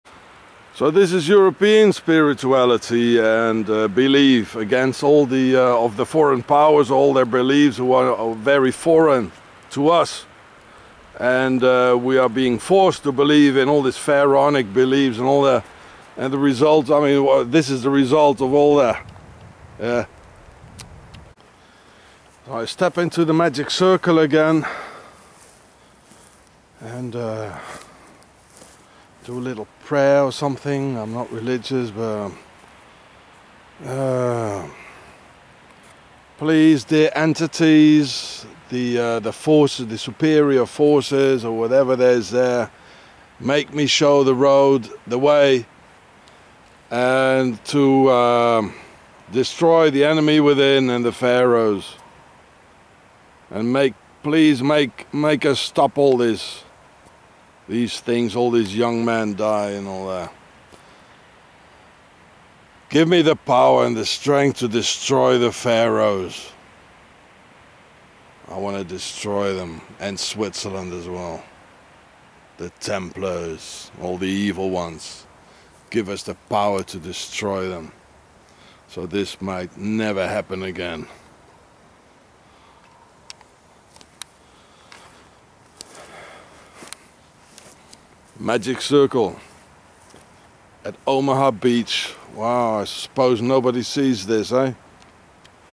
Magic Circle Incantation of Pagan Belief vs. Massmurderous Institution of the Pharaonic Religion
When I came out of the Magic Circle or the Fairy Ring after the incantation the Hawk or the Horus Falcon showed itself to me and my camera (footage in other vid). People worship this invisible pharaonic god of the religious institution, and people die by the millions; this pharaonic Hocus Pocus is a death religion.